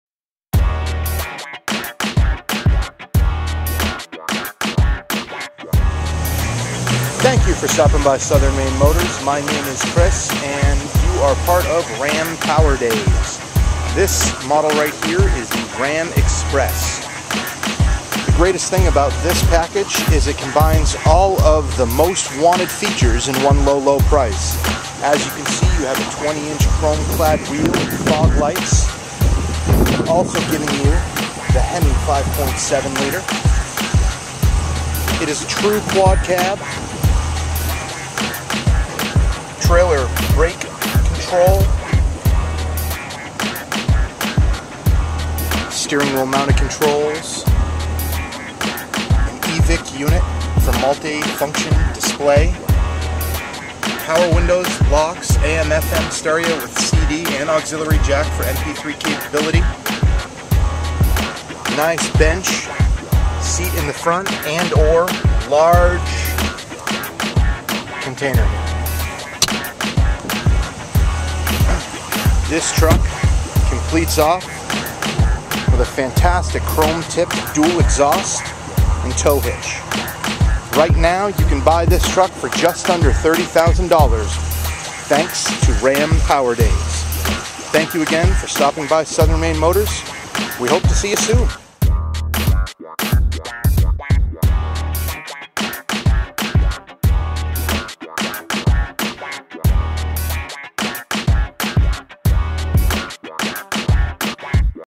Ram Power Days 2012 Ram 1500 Express Southern Maine Motors Saco Maine 04072
Ram Power Days 2012 with this Ram 1500 Express. Chris tell us all about this trim level and why it is such a great package.